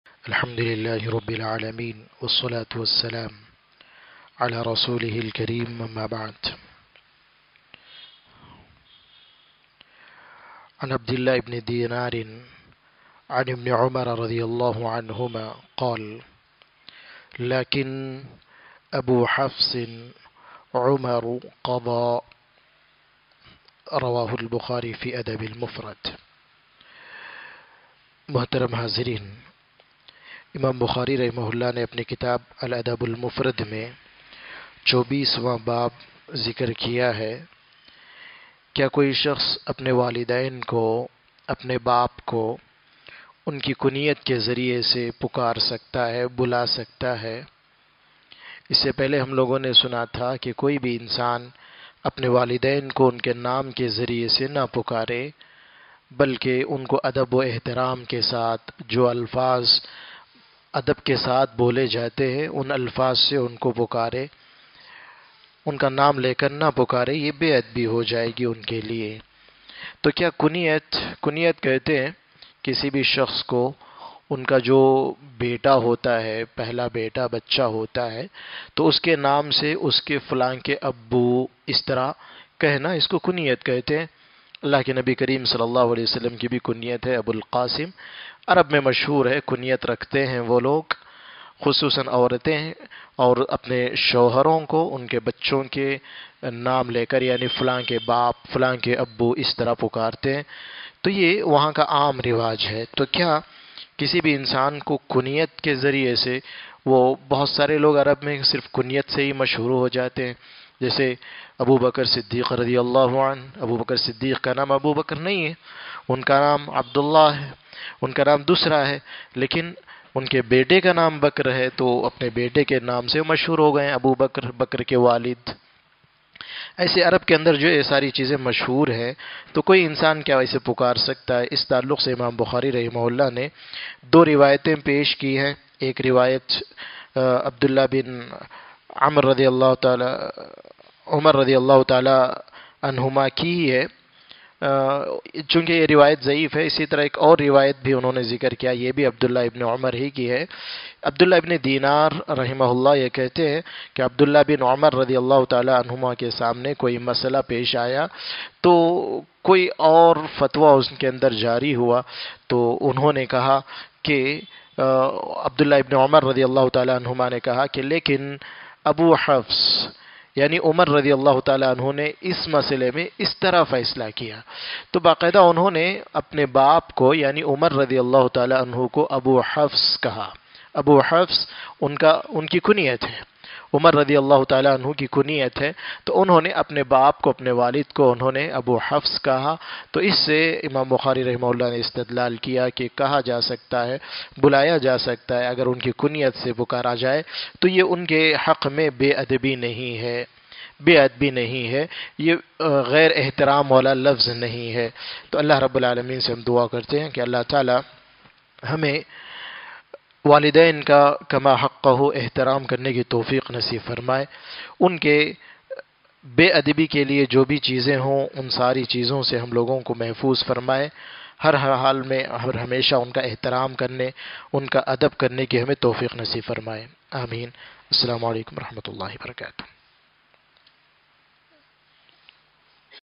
Apne Walidain Ko Unki Kuniyat Ke Zariye Bulana? | Shaykh Maaz Khan Muhammadi
Topic: Apne Walidain Ko Unki Kuniyat Ke Zariye Bulana?

Speaker: Shaykh Maaz Khan Muhammadi

٠٠٠٠٠٠٠٠٠٠٠٠٠٠٠٠٠٠٠٠٠٠٠٠٠٠٠٠٠٠٠٠٠٠٠٠٠٠٠٠